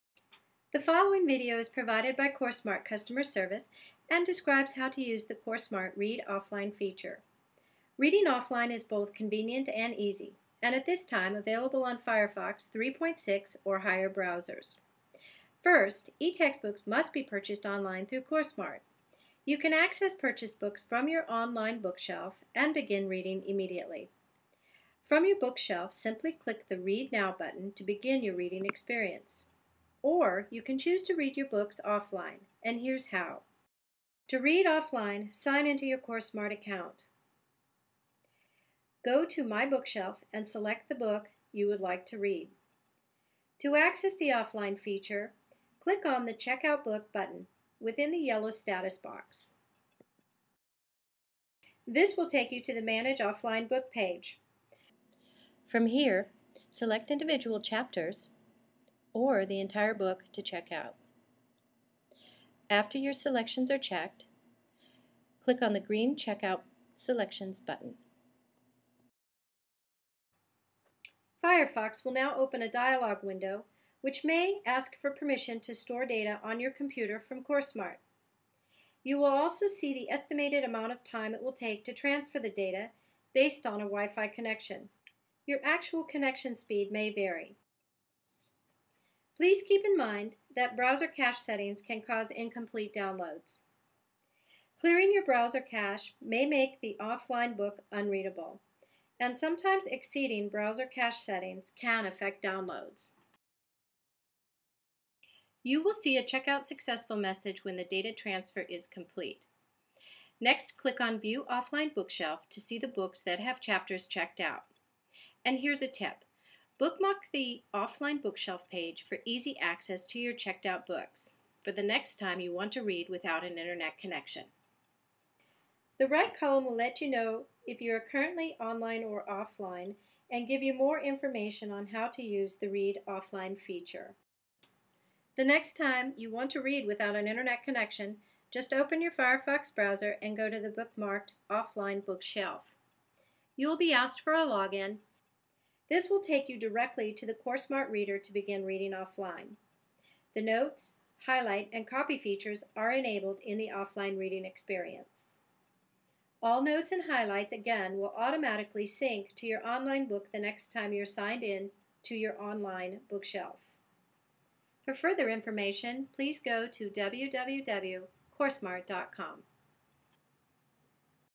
CourseSmart Offline Reading Feature
CourseSmart Offline Reading Feature. A 3 minute video describes how to use the feature to transfer e textbook data to read without an internet connection. Check out by chapter or the entire book to read anytime, anywhere! to find your books.